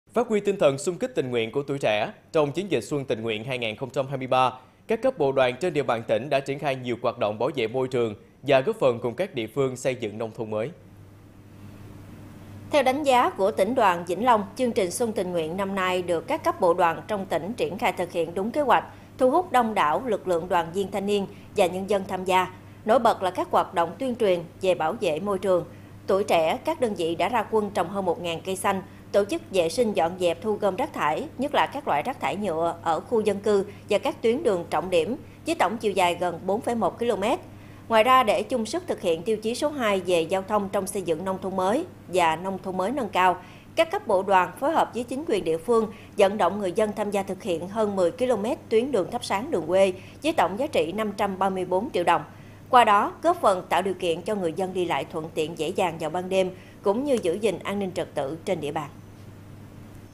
Tuổi trẻ Vĩnh Long trồng hơn 1.000 cây xanh trong chiến dịch xuân tình nguyên 2023
TUỔI TRẺ VĨNH LONG TRỒNG HƠN 1.000 CÂY XANH TRONG CHIẾN DỊCH XUÂN TÌNH NGUYỆN 2023

Phát huy tinh thần xung kích tình nguyện của tuổi trẻ, trong chiến dịch xuân tình nguyện 2023, các cấp bộ Đoàn trên địa bàn tỉnh đã triển khai nhiều hoạt động bảo vệ môi trường và góp phần cùng các địa phương xây dựng nông thôn mới.

⬇️⬇️⬇️ Tải THVL Audio để nghe đầy đủ